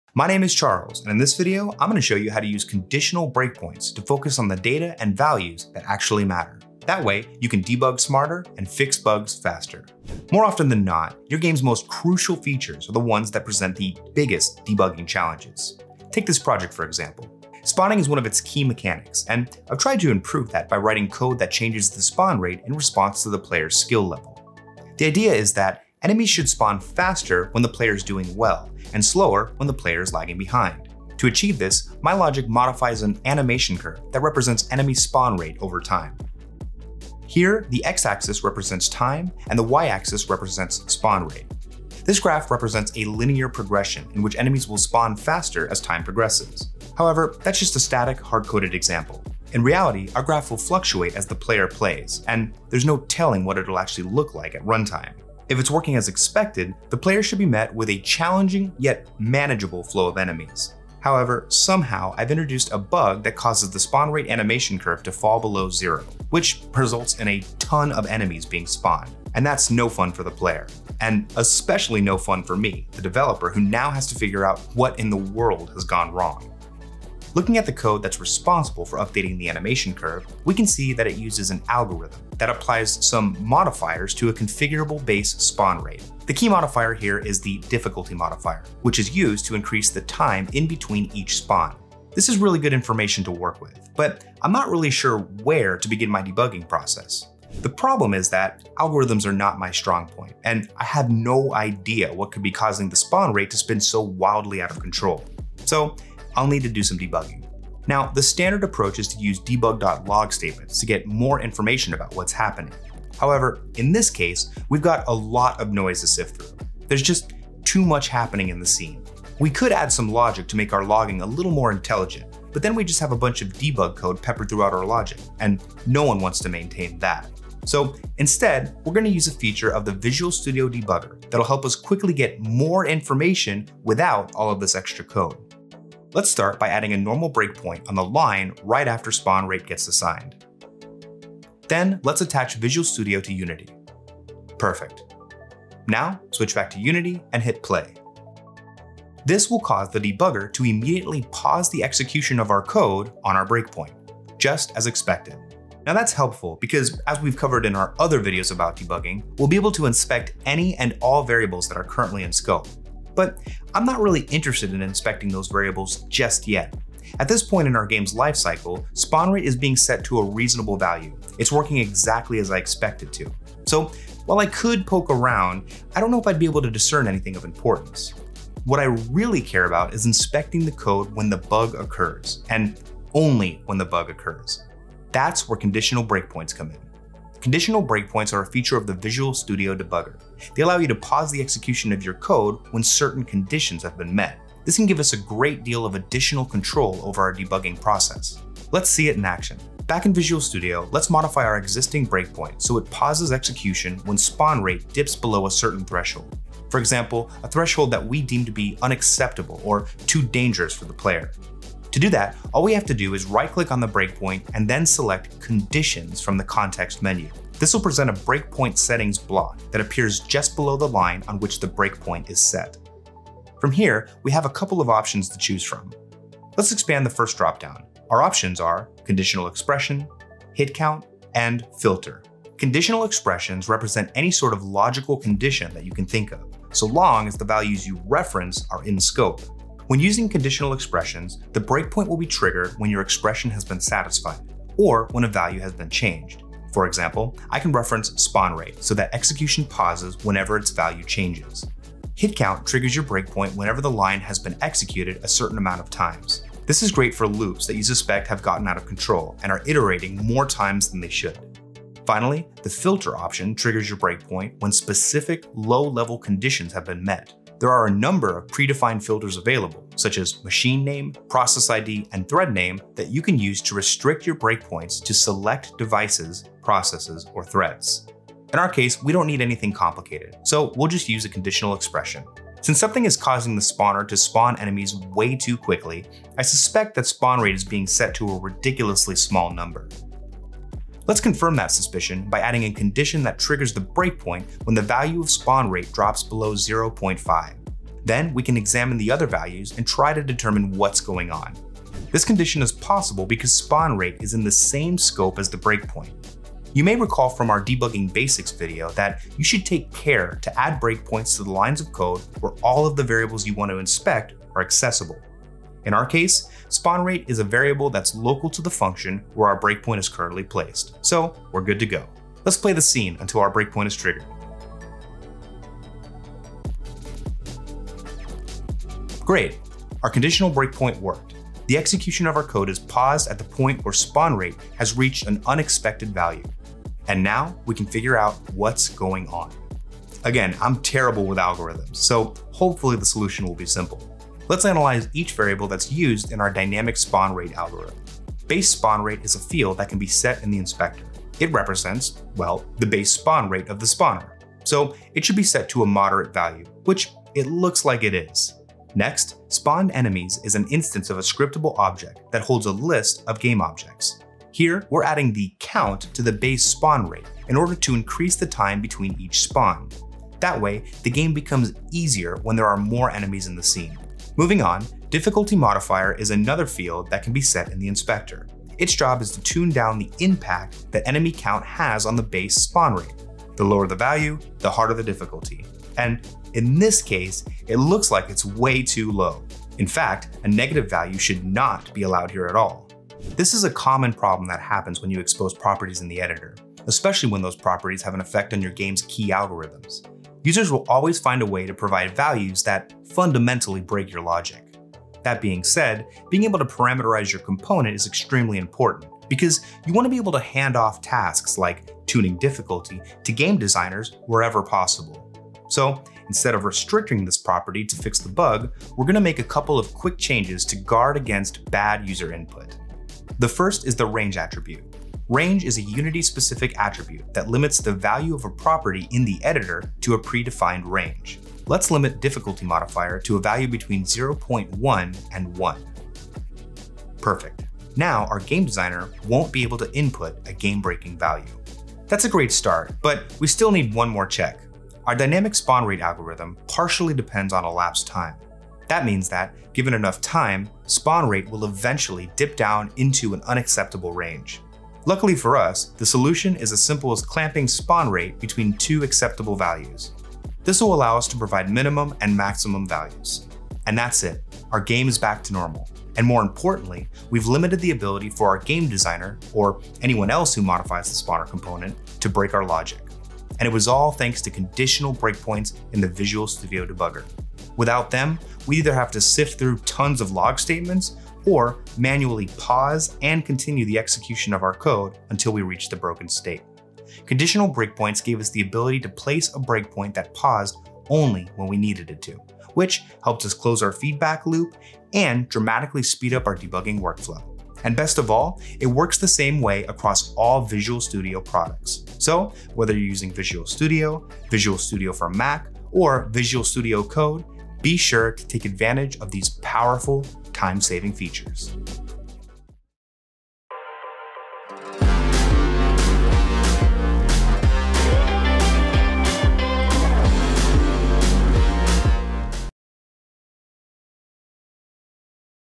Conditional Breakpoints [4 of 5] | Beginner’s Series to: Visual Studio Tooling for Unity Developers
You don't have to put Debug.Log() statements all over your code to know when something is happening. Instead, we can use the debugger to pause our game only when something we suspect might happen. In this short video we'll show you how to stop and inspect what's happening in your games when a certain condition is true using Conditional Breakpoints and the Visual Studio debugger.